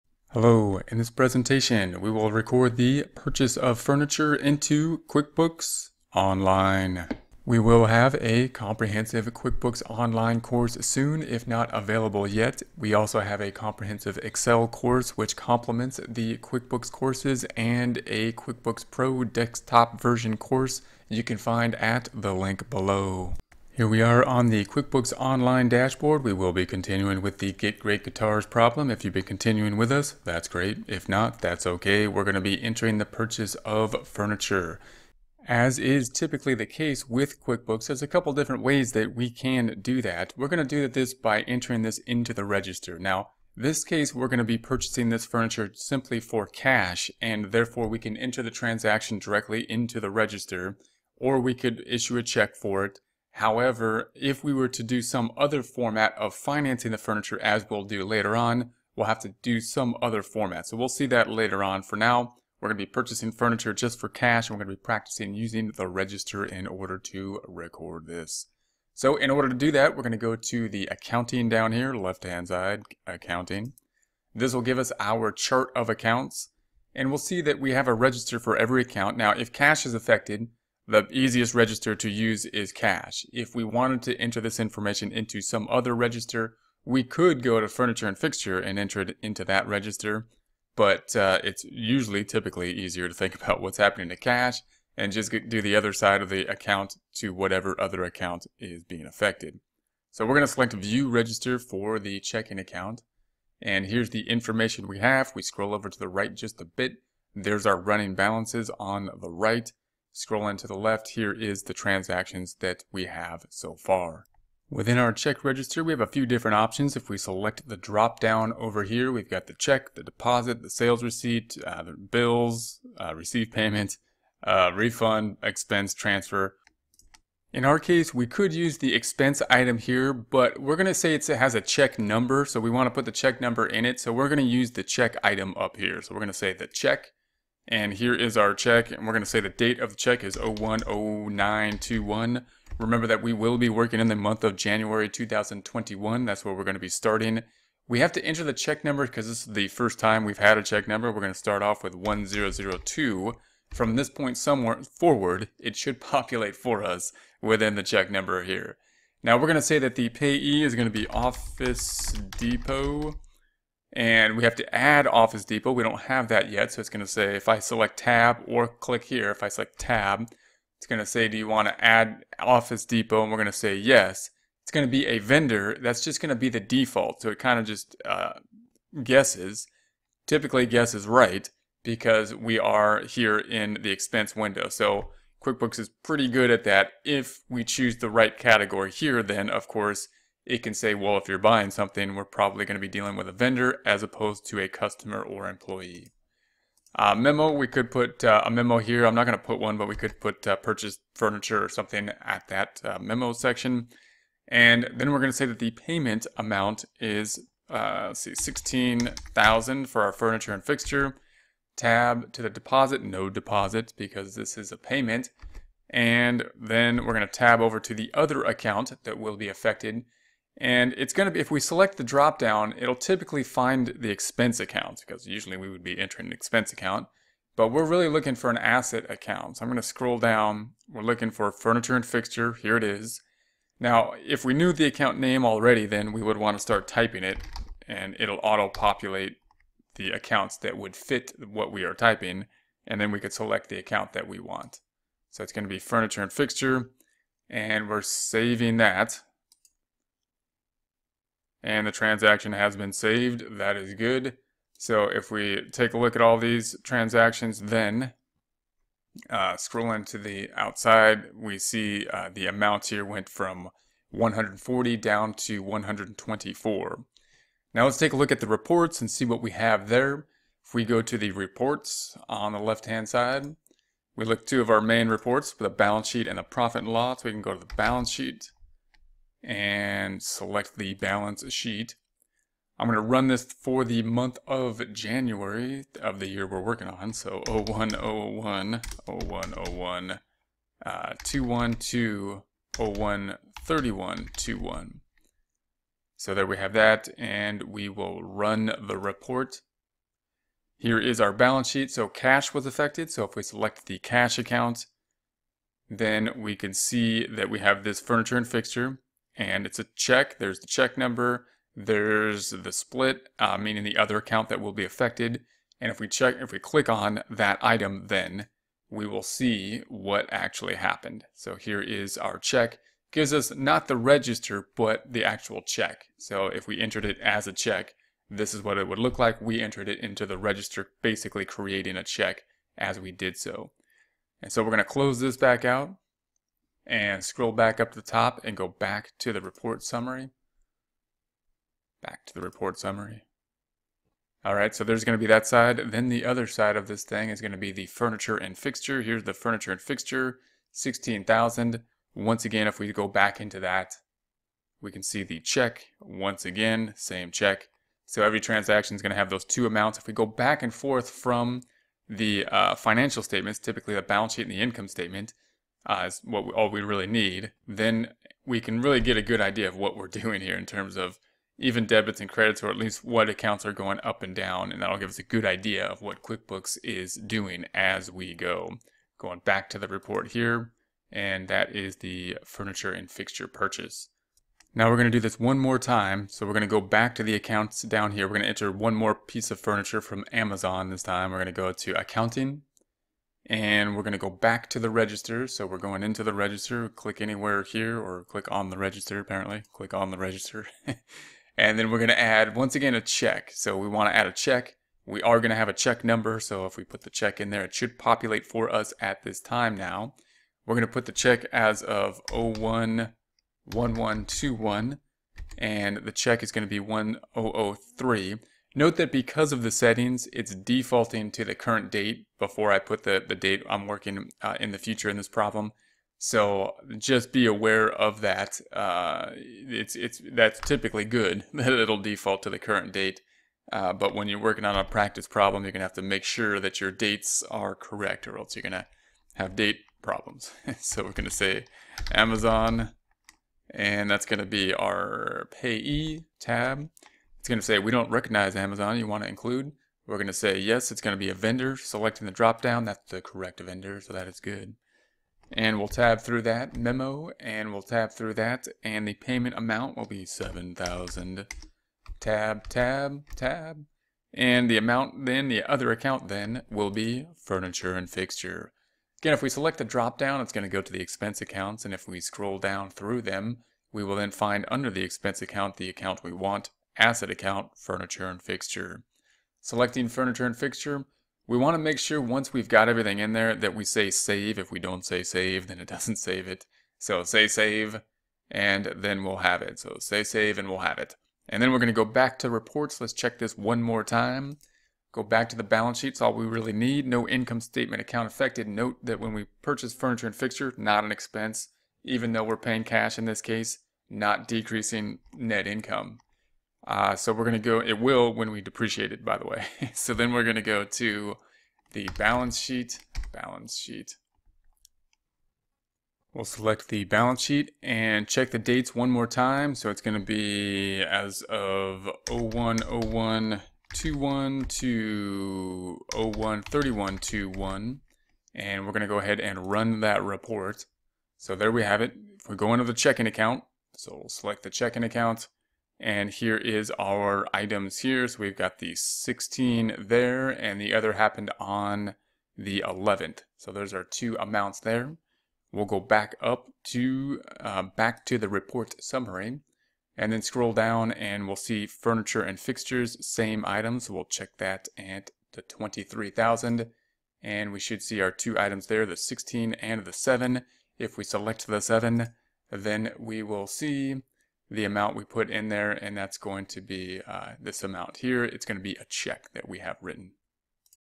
QuickBooks Online 7.10 Part 1 Record Purchase Furniture 2018
We will record the purchase of furniture for a loan into QuickBooks Online 2018. The purchase of furniture is a transaction that is not done on a day to day basis and therefore does not have a set form in the QUickBooks Online system. Because we are not using cash we cannot generally use QuickBooks Online's check register.